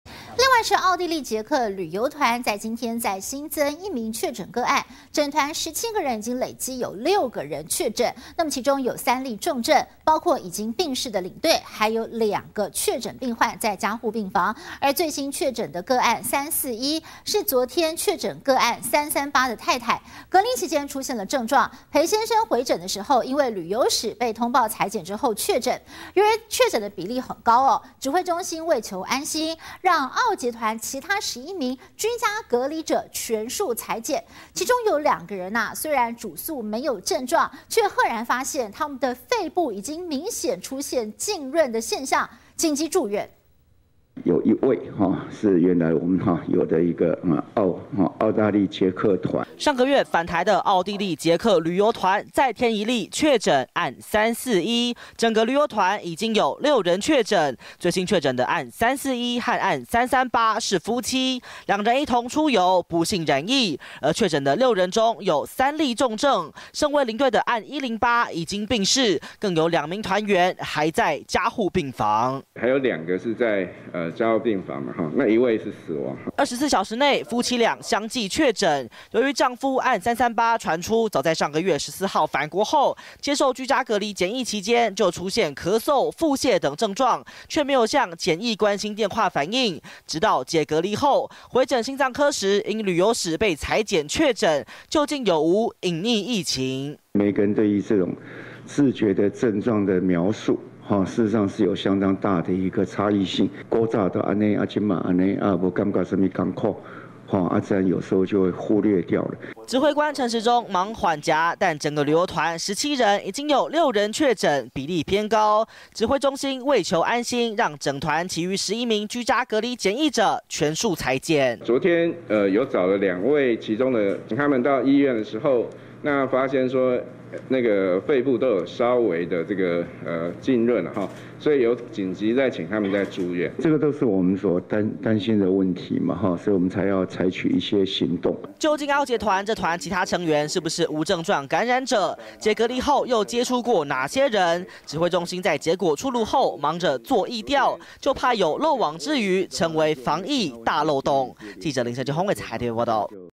案338咳嗽腹瀉未通報 恐涉隱匿重罰百萬
奧地利捷克旅遊團，今天(3號)再新增一例確診，整團17人已累計6人確診，其中有3例重症，包括已經病逝的領隊，還有2名確診者在加護病房。而最新確診個案341，是昨天(2號)確診個案338的太太，隔離期間出現症狀，陪先生回診時因旅遊史被通報採檢後確診。由於確診比例高，指揮中心為求安心，讓奧捷團其他11名居家隔離者全數採檢，其中2人雖主訴沒有症狀卻被發現肺部明顯浸潤已緊急住院。